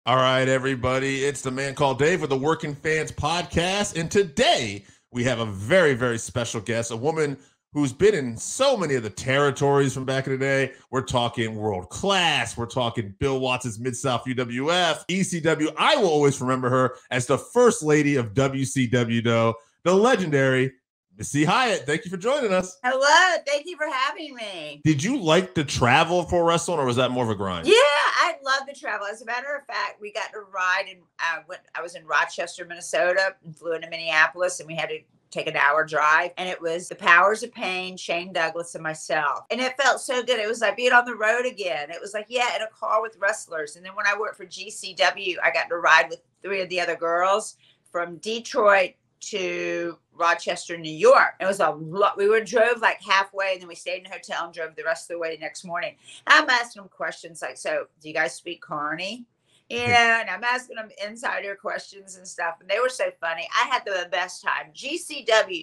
Does Missy Hyatt enjoy travelling for wrestling?
The Man Called Dave sits down with Missy Hyatt (Missy Hyatt on Facebook, therealmissyhyatt on Instagram, @missyhyatt on Twitter, @missyhyatt1stladyofwrestling ) to talk about whether Missy enjoys travelling for wrestling. Enjoy the interview and enjoy your holiday!

Try the best new hard seltzer/sports drink combo with electrolytes over at drinkgameup.com.

Facebook-Working Fan’s Podcast